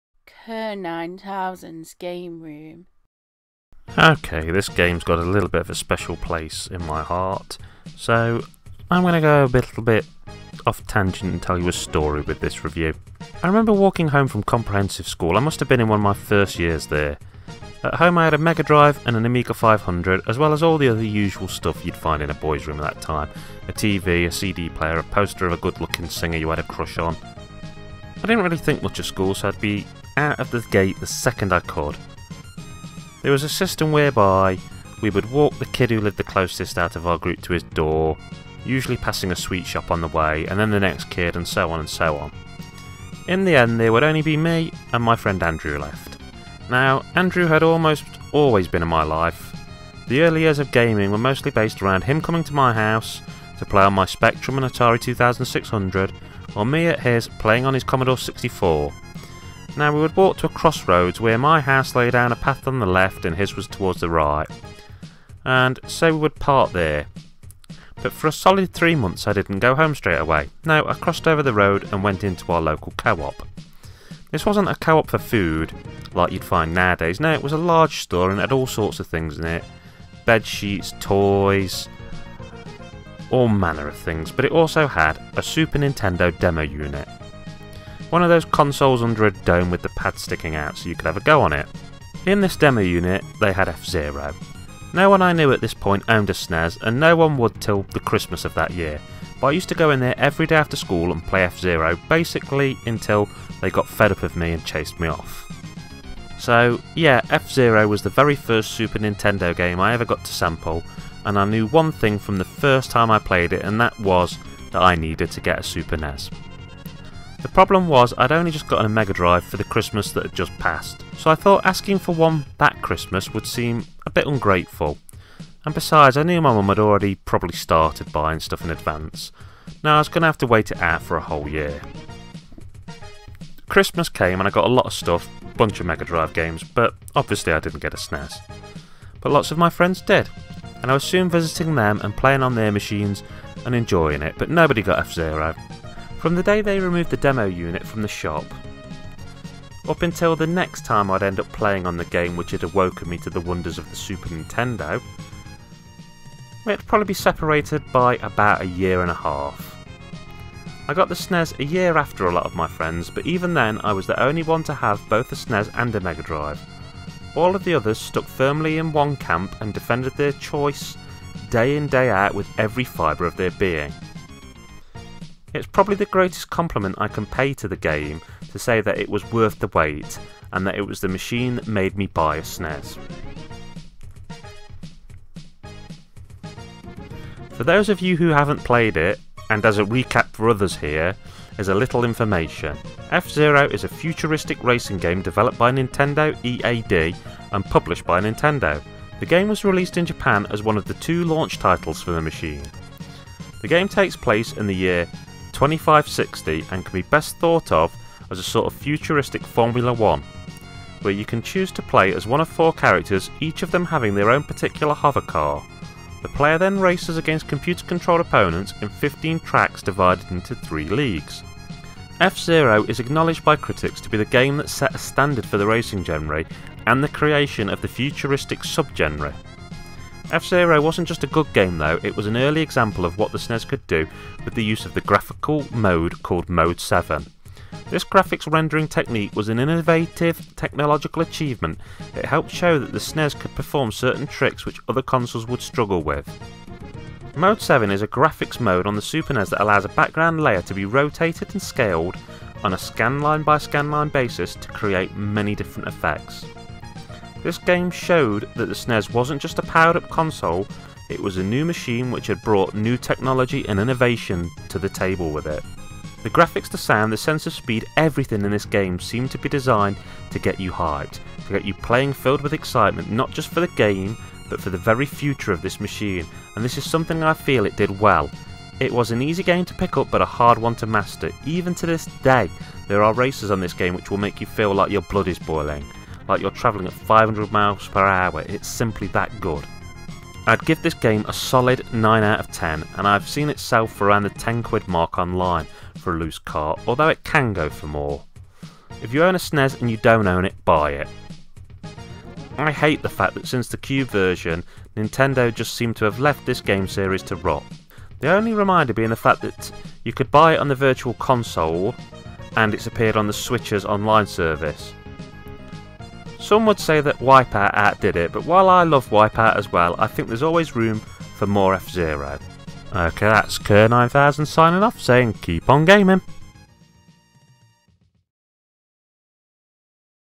My F-Zero Super Nintendo Review
My SNES Review of F-Zero part of my 150 SNES game review series